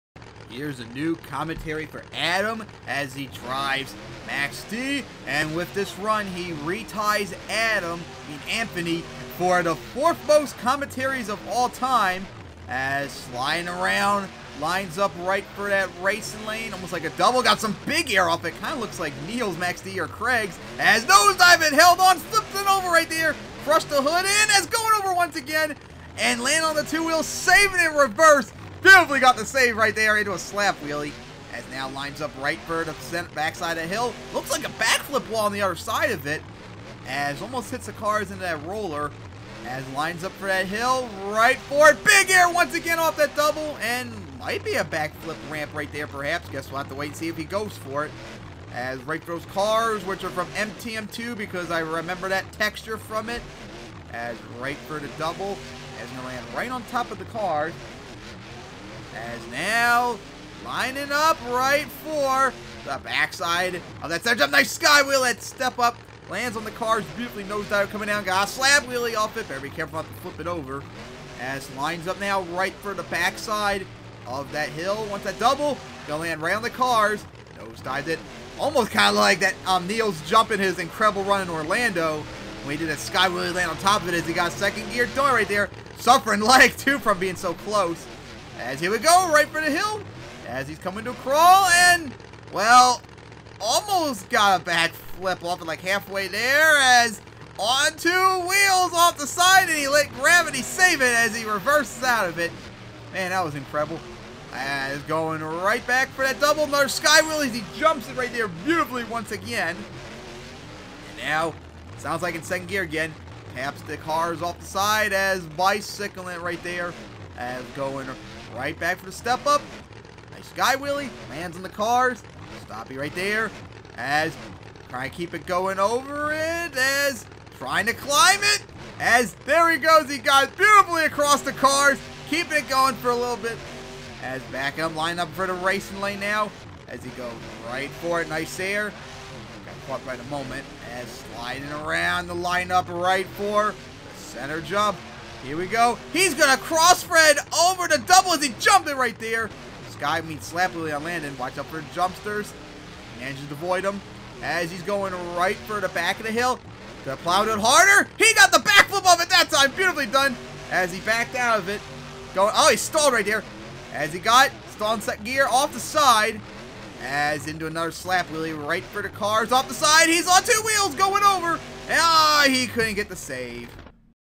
RoR Monster Jam Freestyle Commentary #1,427 (ABGamerX)
Another awesome run by Adam!